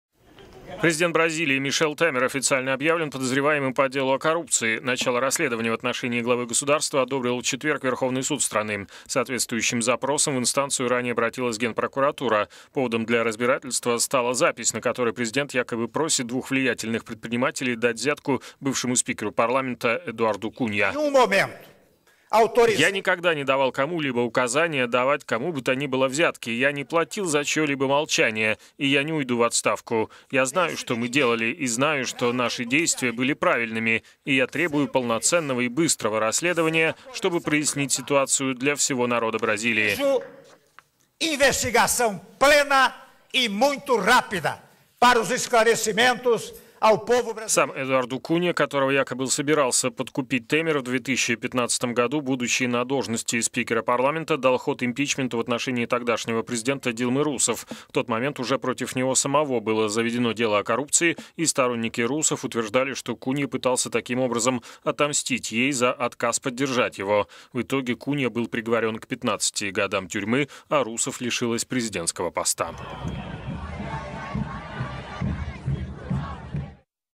Верховный суд объявил президента подследственным
Президент Бразилии Мишел Темер официально объявлен подозреваемым по делу о коррупции. Начало расследования в отношении главы государства одобрил в четверг Верховный суд страны. С соответствующим запросом в инстанцию ранее обратилась Генеральная прокуратура. Поводом для разбирательства стала запись, на которой президент якобы просит двух влиятельных предпринимателей дать взятку бывшему спикеру парламента Эдуарду Кунья.